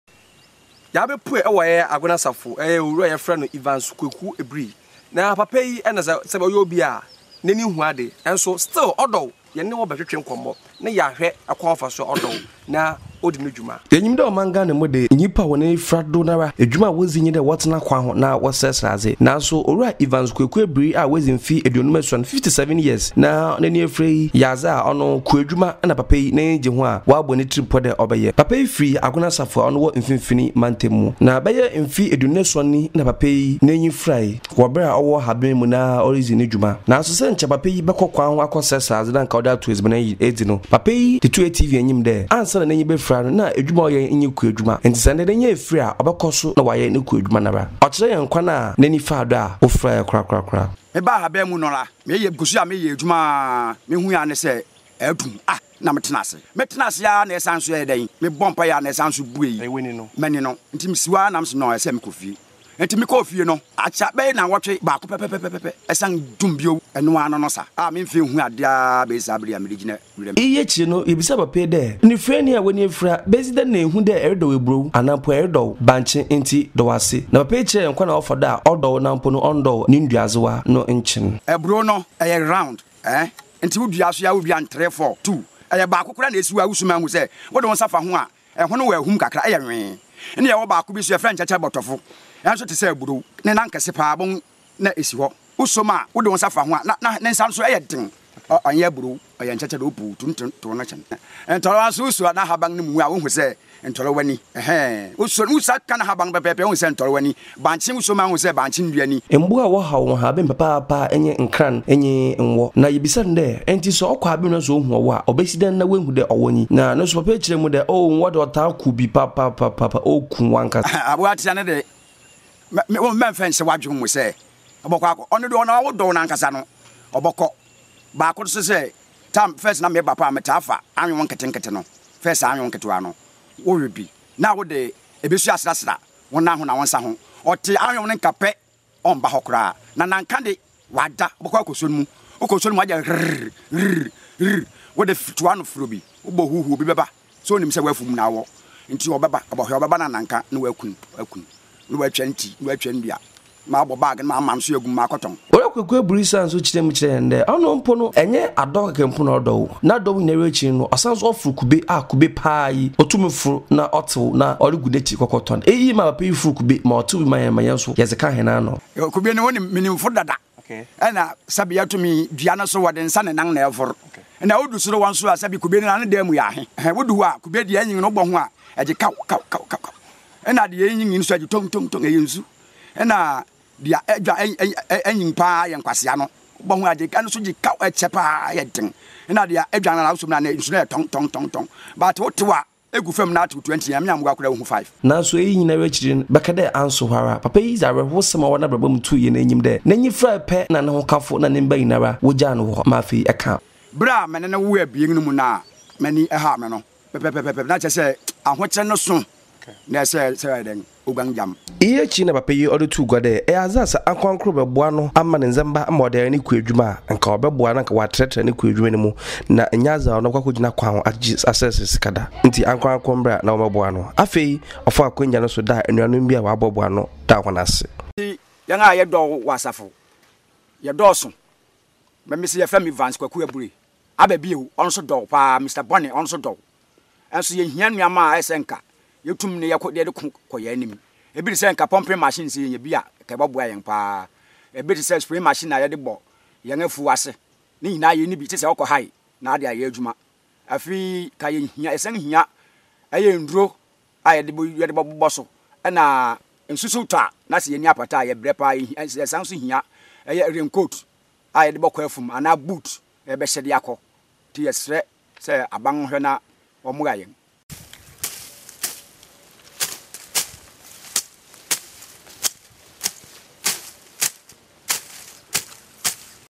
Story of The Week: A blind 57 year old farmer still carrying on.
He is blind, but that did not stop him from working hard.
He is a 57 year old farmer called Evans Kweku Eburi from Agona Asafo.
He has been blind for 25 years now and still does his farm work despite his blindness.
Inspired yet? Go chase your dreams! Do not let that 'disability' stop you!